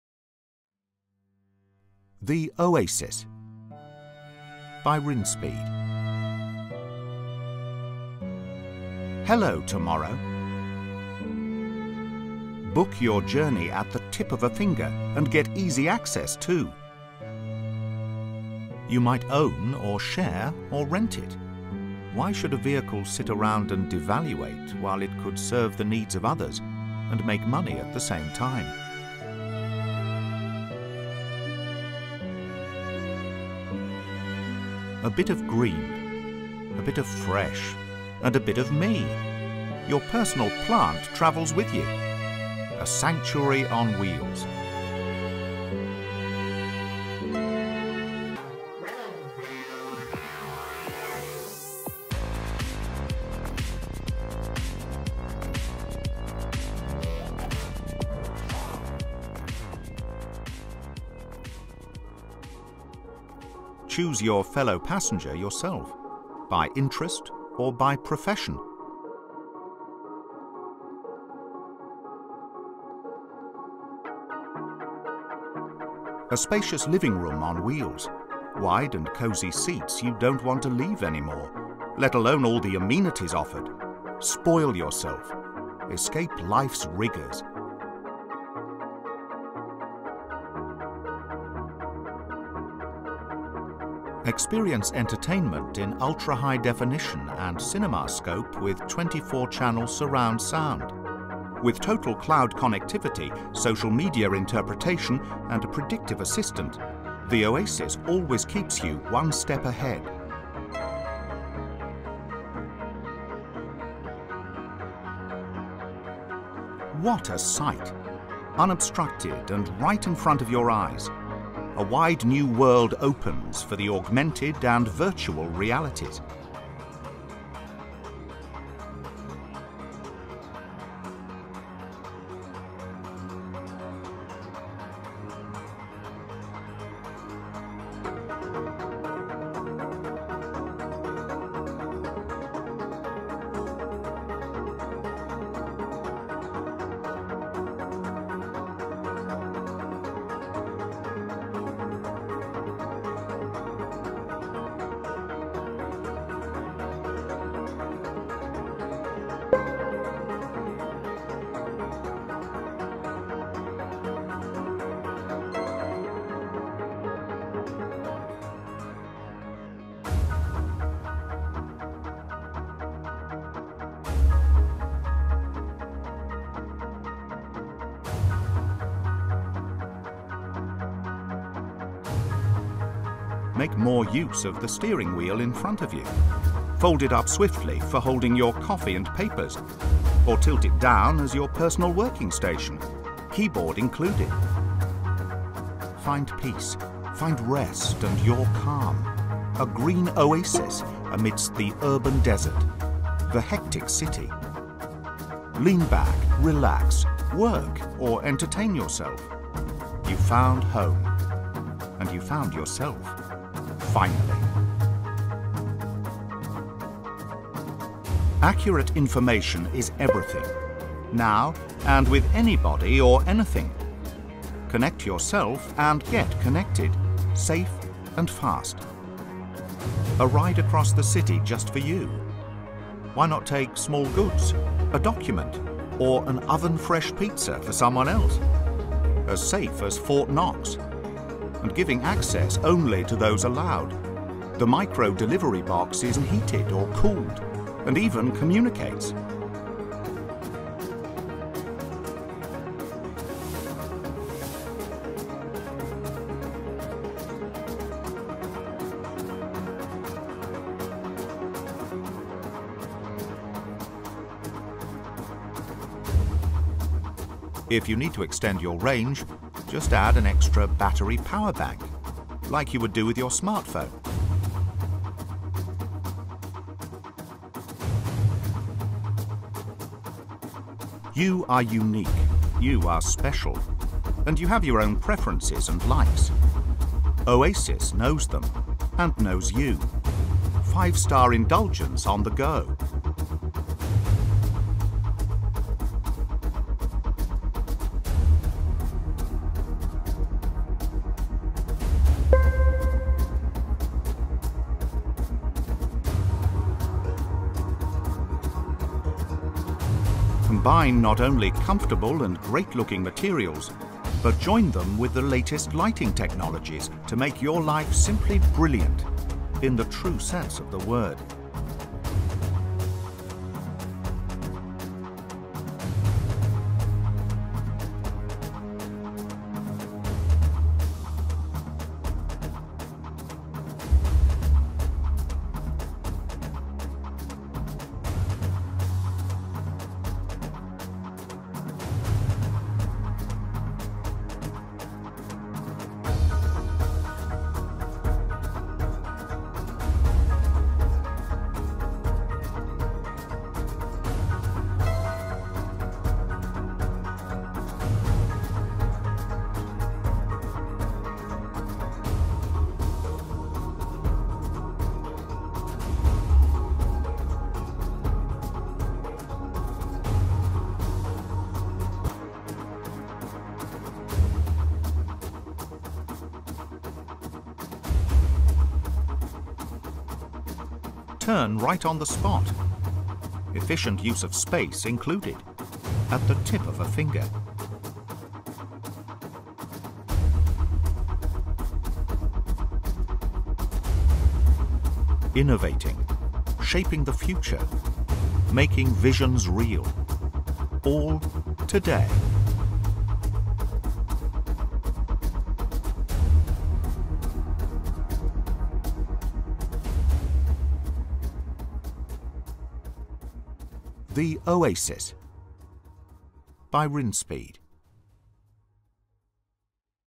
OSRAM Technology Illuminates Rinspeed Oasis Concept Car
Experience the driverless road with Oasis, a concept vehicle from Rinspeed, illuminated by OSRAM Opto Semiconductors. Originally introduced at the 2017 CES, the car boasts safety, connection and communication through tech, transforming the drive into life. For more information on Oasis, visit our